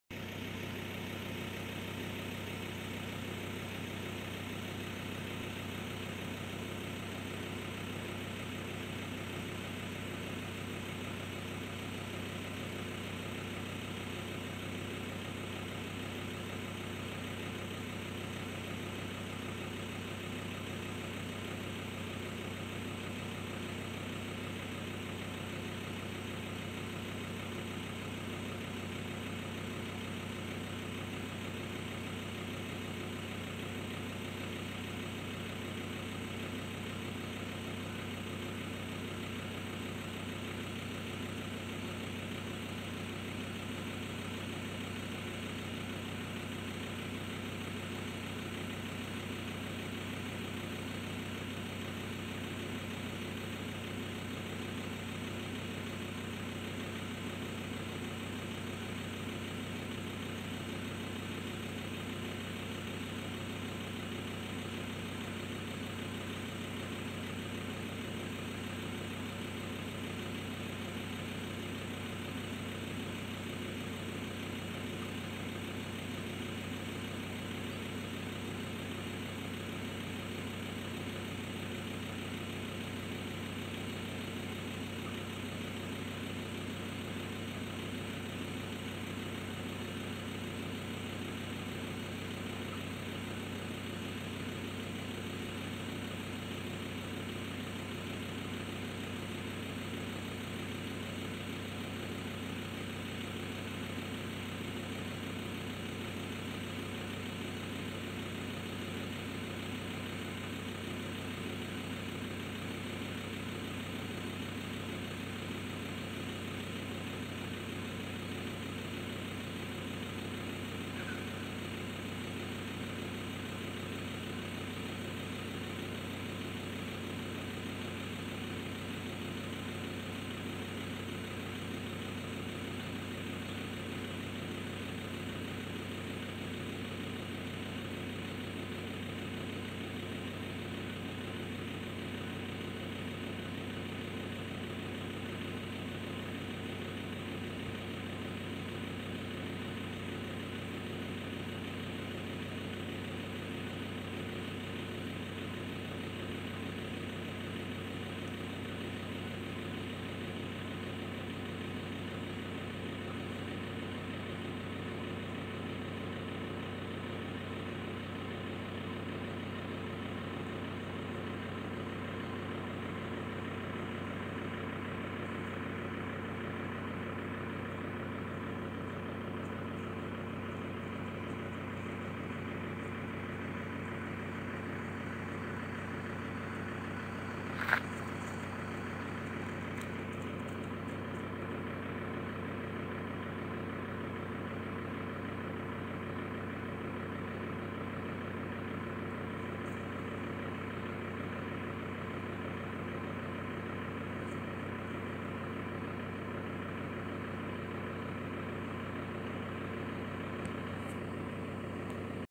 Wombat runs away at Newnes in the Wolgan Valley Newness
Taken around dusk. Saw 10 wombats along the river, trails and roads. Wait for the end ... When the wombat hears me ...